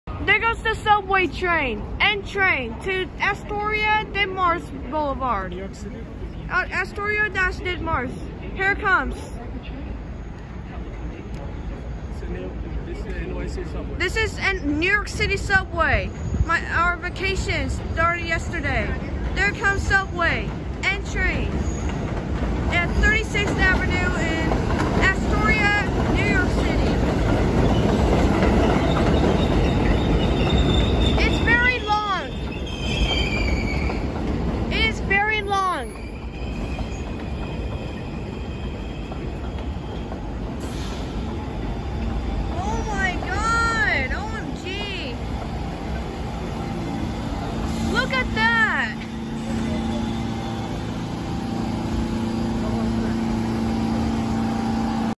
Subway N train at 36th Avenue in Astoria, NYC
When we go to circle line for Manhattan tour, we took the subway N and 7 train. I weren’t able to capture the 7 train, because someone might push me through the train line